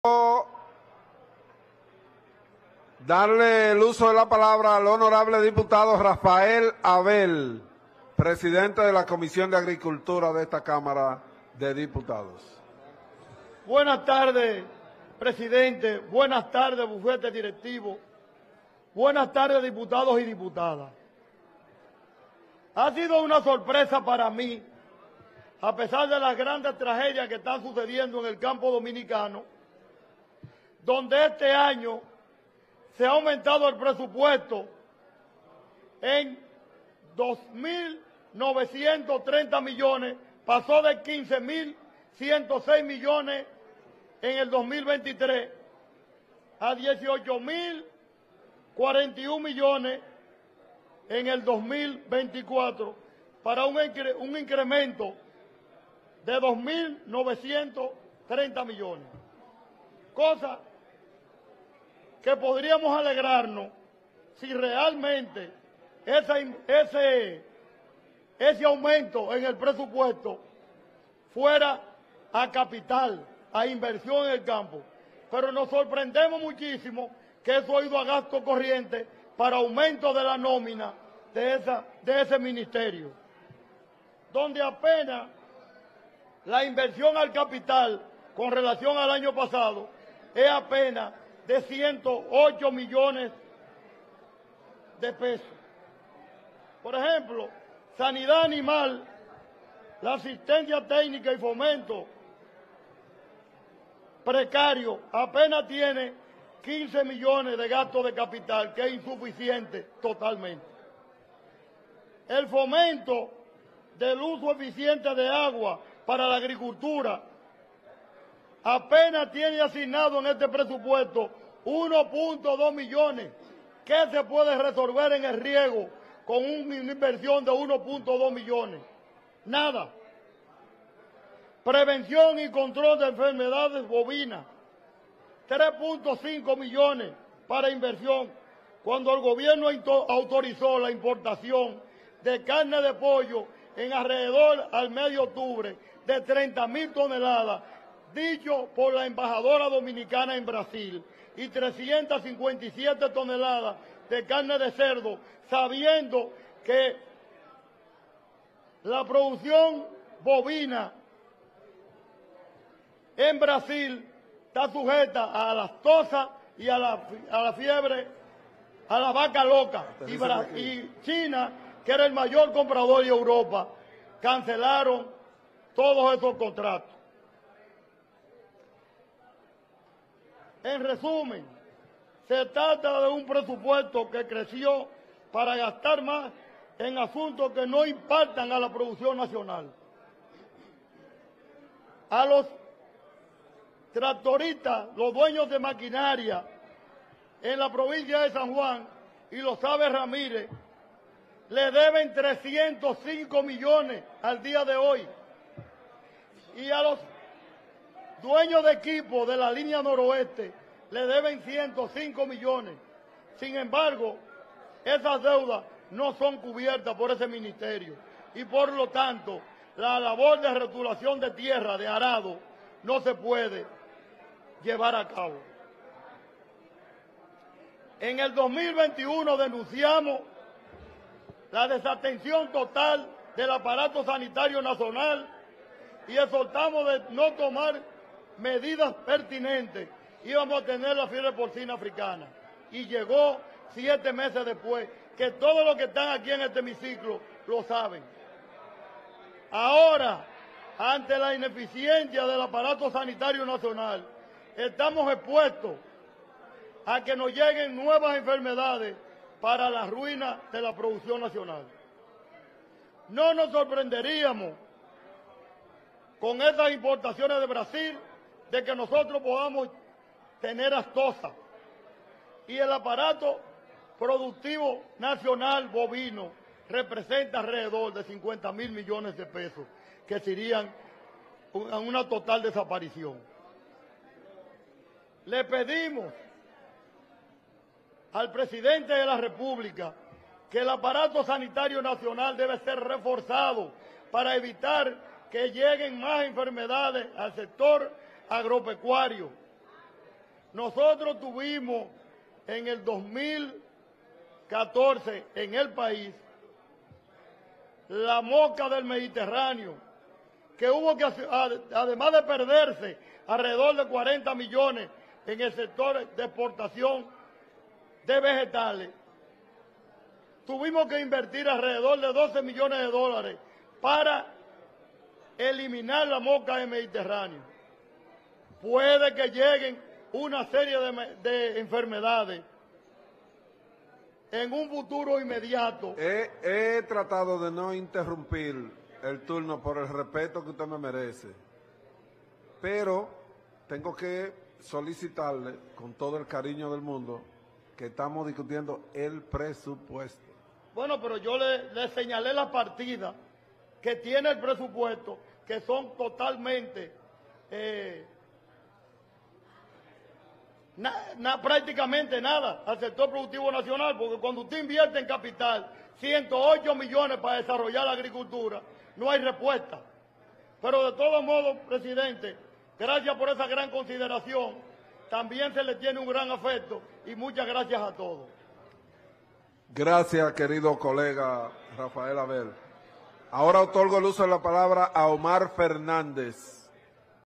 Turno previo del Diputado Rafael Antonio Abel, Sesión No. 00027 martes 12 de diciembre 2023
El diputado Rafael Antonio Abel expresa que este año ha sido incrementado el Presupuesto General para el 2024, pero que no se ve reflejado el aumento en los sectores de la agricultura que lo necesitan tras los impactos de la naturaleza que han escaseado la materia prima de la agricultura del país.